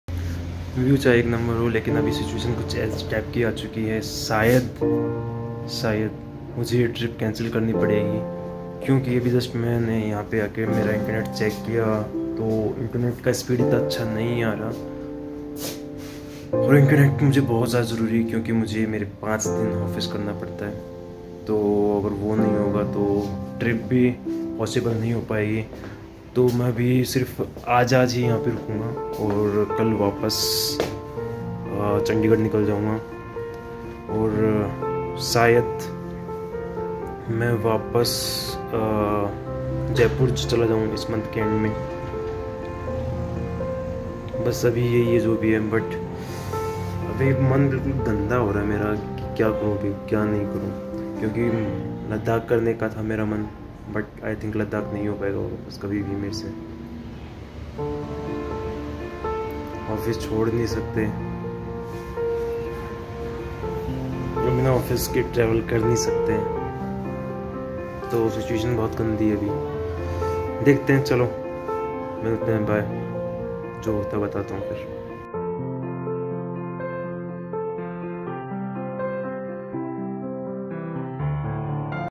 Unexpected Turn: My Ladakh Ride Got Canceled | Here's What Happened 🏍️❌| BeingRoyal
Life on the road is full of uncertainties and this time, my much-anticipated Ladakh bike ride hit an unexpected detour. In this video, I'll be sharing what led to the cancellation of the trip, how I felt, and what's next on the horizon. Sometimes, even the best-laid plans don't go as expected, but every twist and turn comes with its own lesson.

📍 In This Video:

The excitement leading up to the Ladakh journey 🏍️🗻
The unexpected challenges and the ultimate decision ❌
How to deal with disappointments as a traveler 🌍
Planning and preparing for uncertainties on the road 🛣️
A sneak peek into what’s coming up next on the channel! 🔜
It's all a part of the journey! I appreciate all your support and understanding. Let's take this moment to reflect, adapt, and move forward with even more determination.